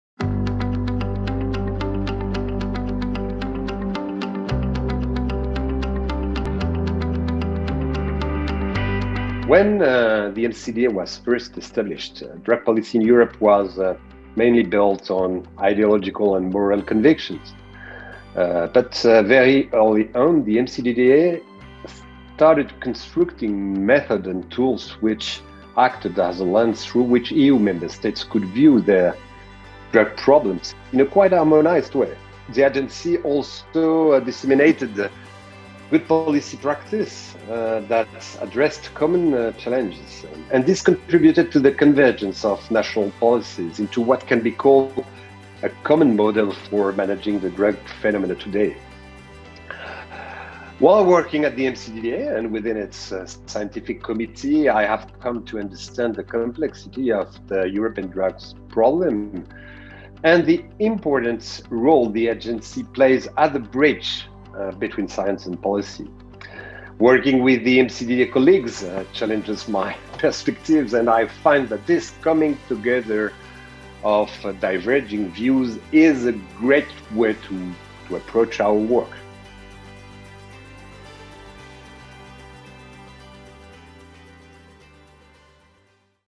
EMCDDA Voices: Henri Bergeron
To celebrate our ‘25 years of monitoring’ milestone, we invited our team and wider community to join us on a digital storytelling journey. Today, Vice-Chair of our Scientific Committee Henri Bergeron tells us what the EMCDDA means to him emcdda25.